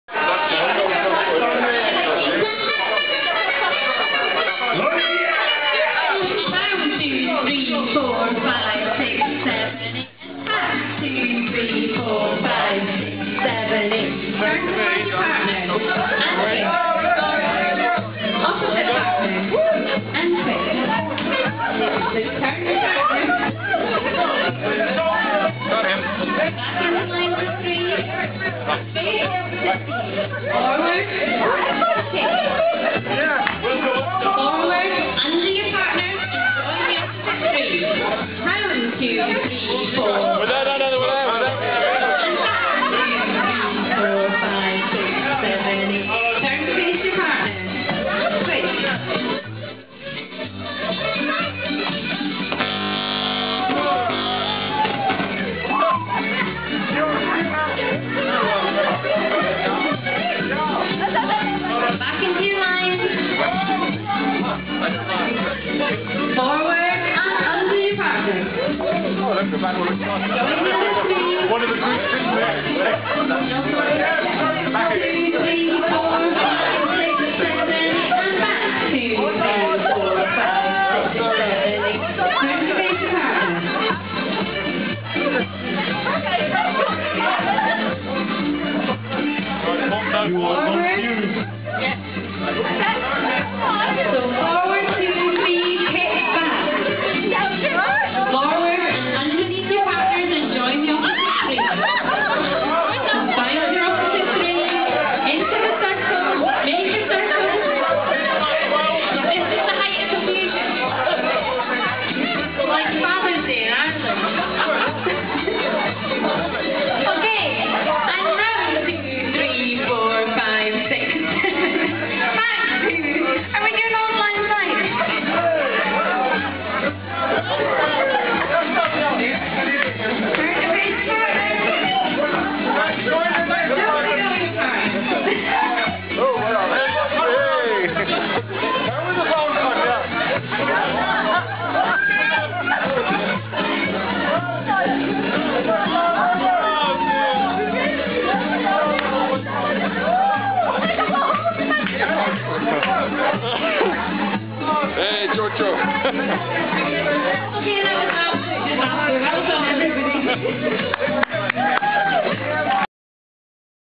GSE D1020 10 Nov09 D.3GP
GSE D1020 Scotland visit to D9320 Odendaalsrus South Africa - Scottish theme evening dancing D